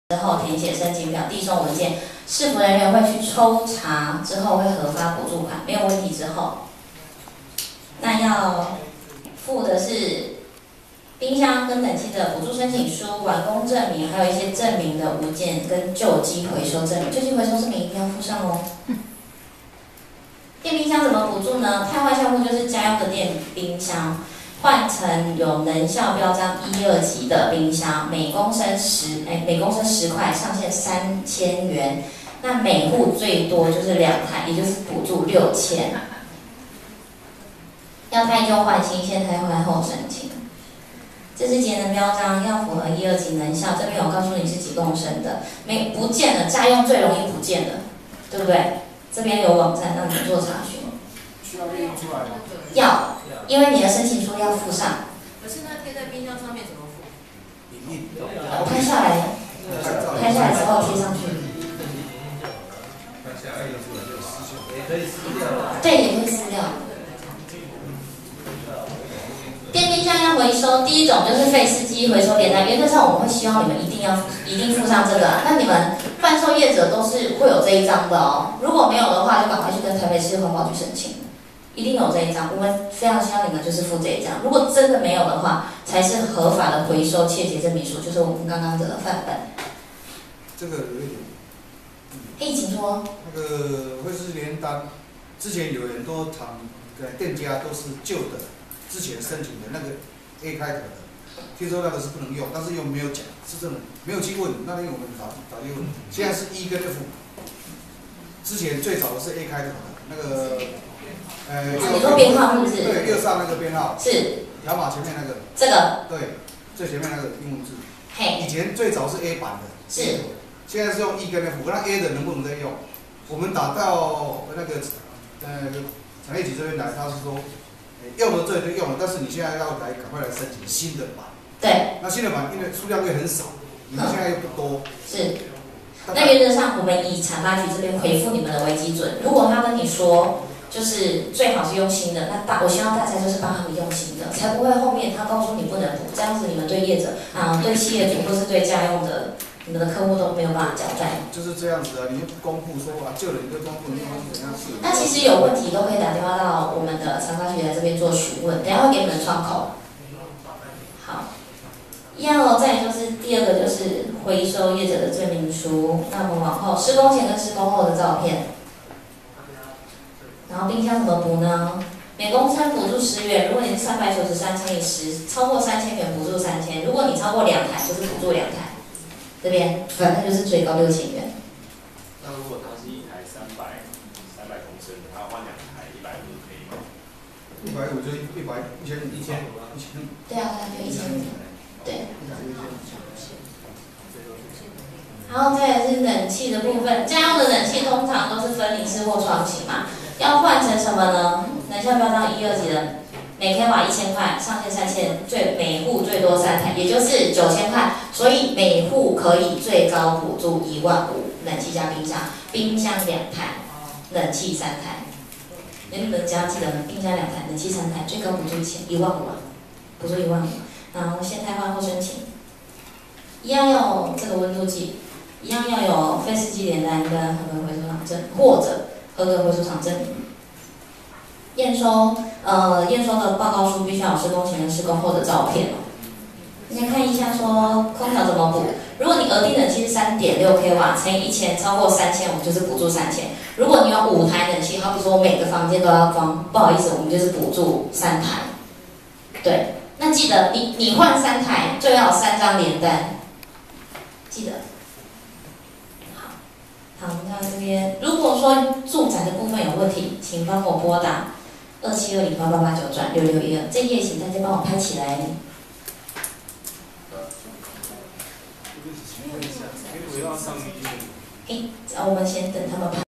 108年 台北市節能設備補助說明會
針對今年節能補助說明會，中央空調系統，電冰箱，能源管理系統，空氣調節機，照明燈具，室內停車場智慧照明，都放寬申請辦法，都有專案專人協助辦理